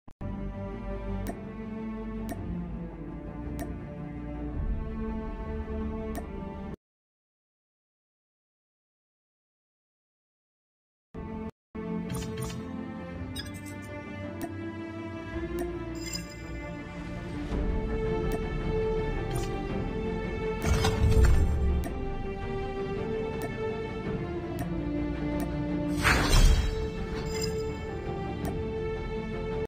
FREE 3 SILVER CRATE COUPONS: JUST USE THIS CODE!
Here's a new redemption code for you to receive a reward. To claim it, head to the Game Lobby, select "Events," "Mission," "Special" and finally click on the "Redemption Center" to enter your code!